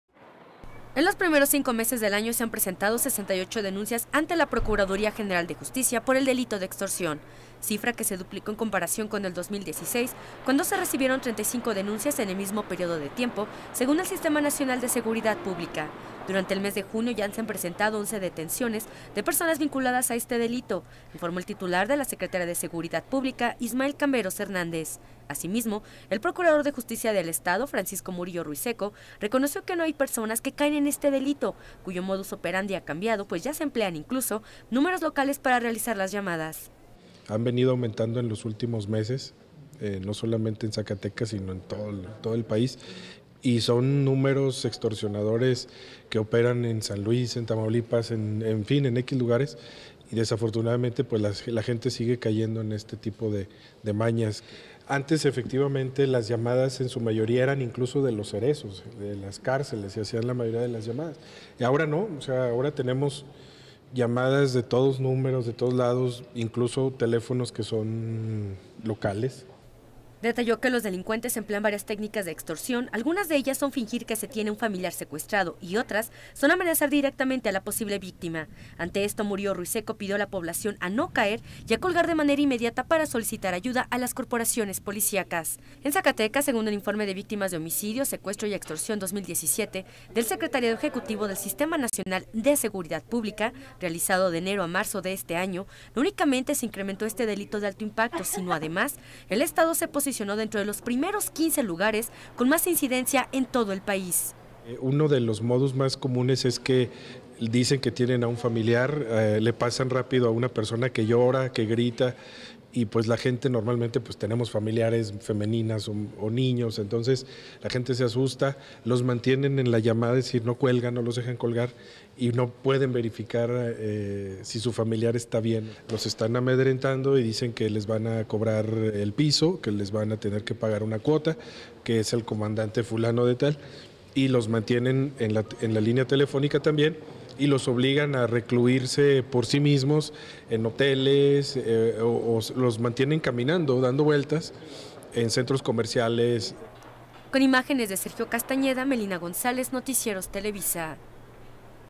AUMENTA EL DELITO DE EXTORSIÓN
68 Casos de extorsión de enero a mayo.